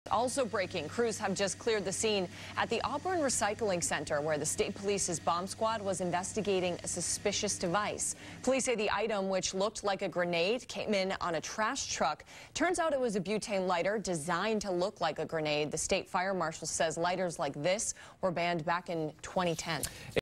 Grenade-Shaped Lighter Prompts Bomb Squad Response At Auburn Recycling Facility
Lighters like the one involved were banned in 2010.